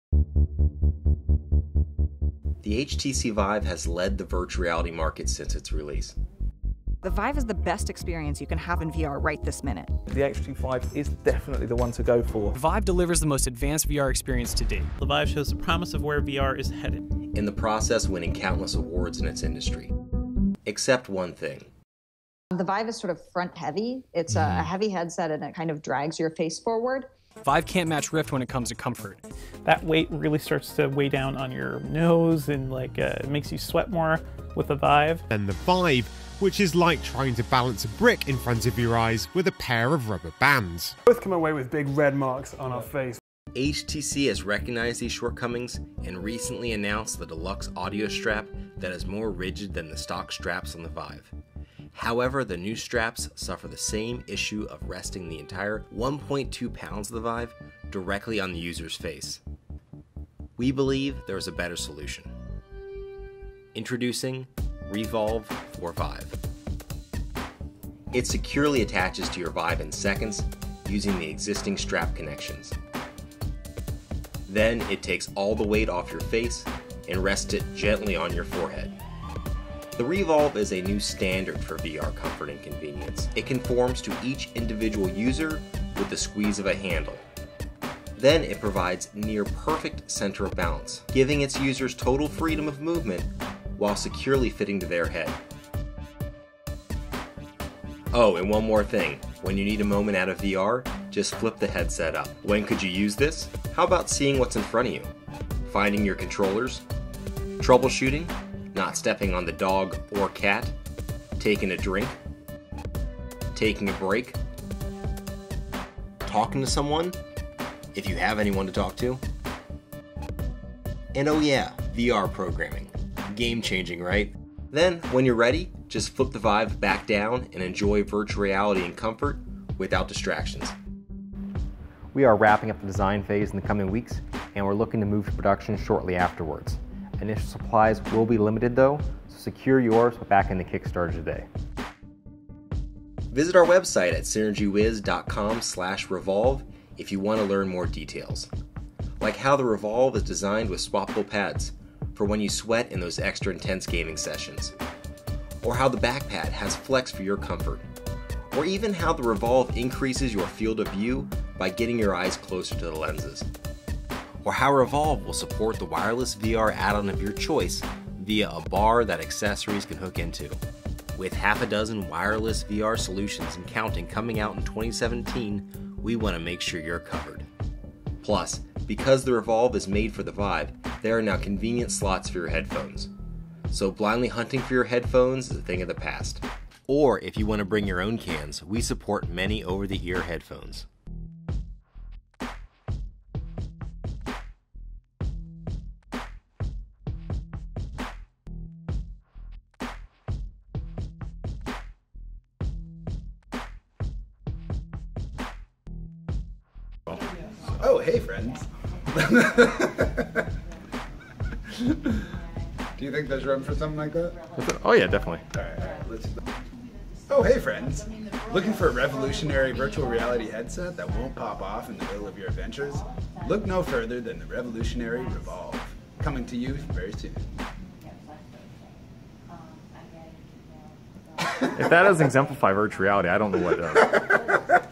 rEvolve Kickstarter Promo
Do you wish Virtual Reality was easier and more comfortable to use?

Now it is with rEvolve!
rEvovle is a new Vive accessory that provides a flip up display, perfectly centered weight distribution, and fits with a single grip. So putting on and wearing your Vive (Virtual Reality Headset) will be as effortless as wearing a baseball cap. Plus, you will enjoy more immersive VR with the increased field of view, and a hassle free audio experience now that the headphones are routed through the rEvolve.

Amazingly the rEvolve does all this by simply replacing your current Vive straps.